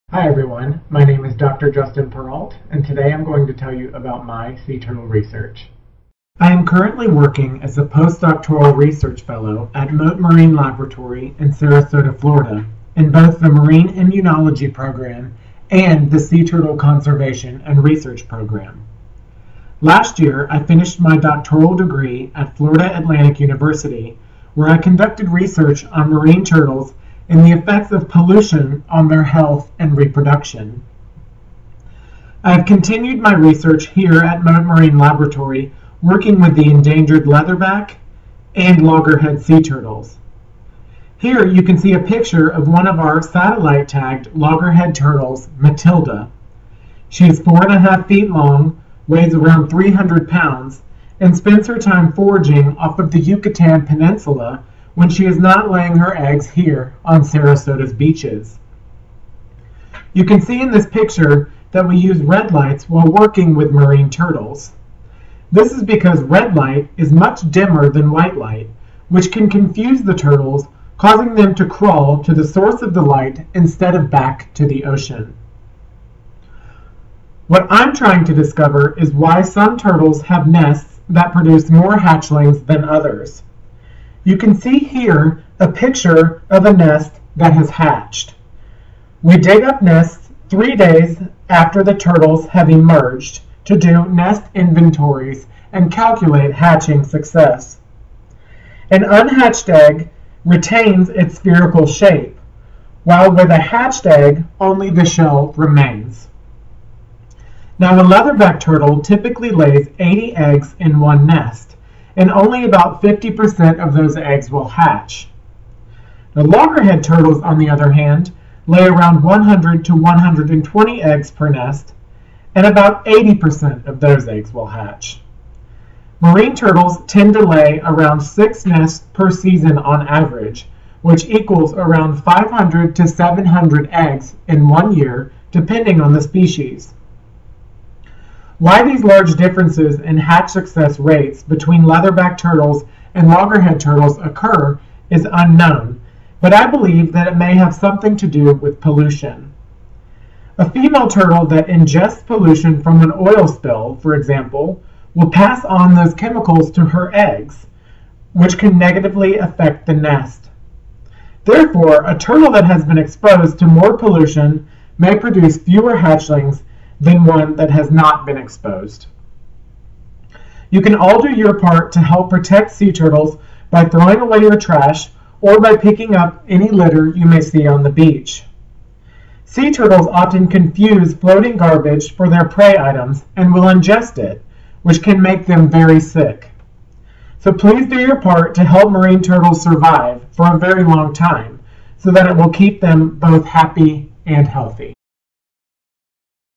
Sea Turtle Research at Mote Marine Laboratory
Meet Dr. Perrault, former educator at the North Carolina Aquarium at Fort Fisher. Learn about his research on sea turtles at Mote Marine Lab.